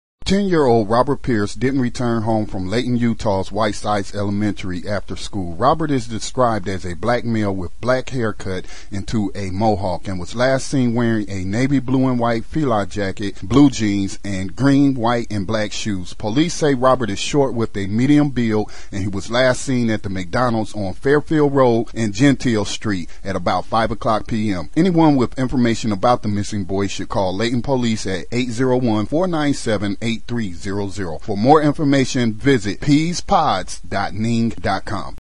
Robert Pierce IV - HAS BEEN FOUND ALIVE
Riyla Alert was canceled as the young man has been found alive.